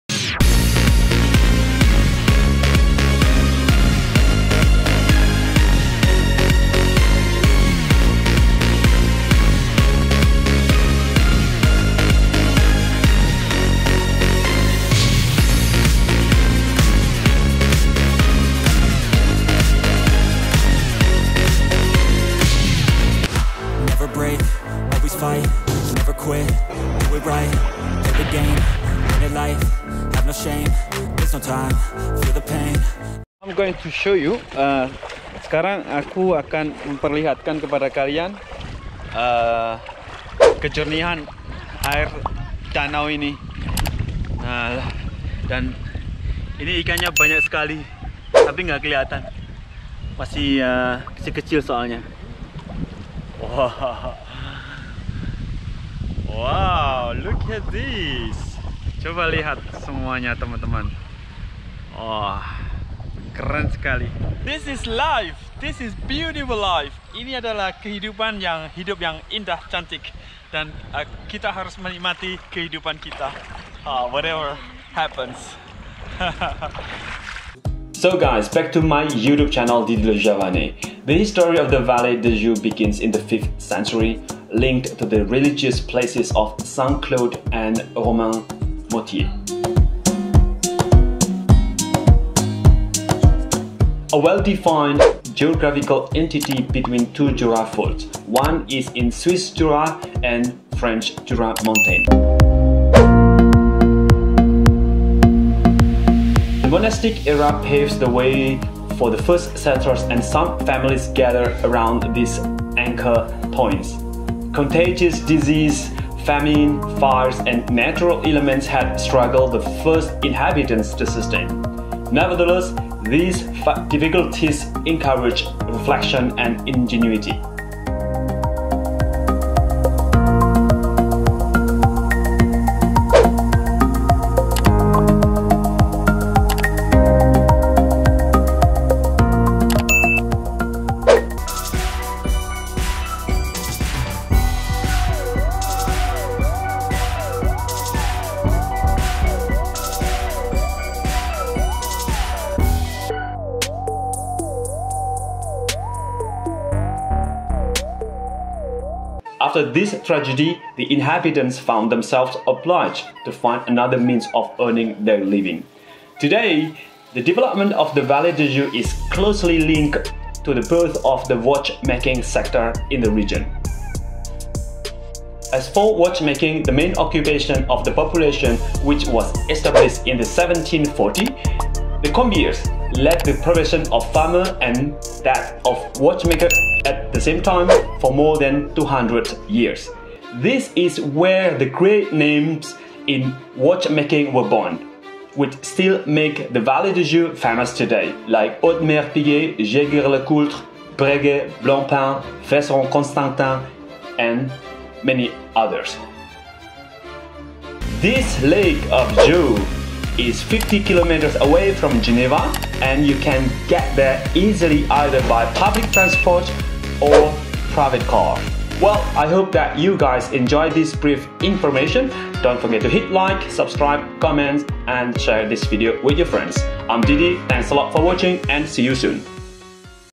This is where the lux watches were born in Switzerland
Hello there, it’s been a while though!!

Hope everyone is doing fine.

Anyway, this video shows you the following points:
1. A brief story of vallee de joux
2. The earning source of income
3. How to get there from Geneva

I hope that this video pleases you and enriches your general knowledge.

Thanks a lot for watching and see you soon.

Didi Le Javanais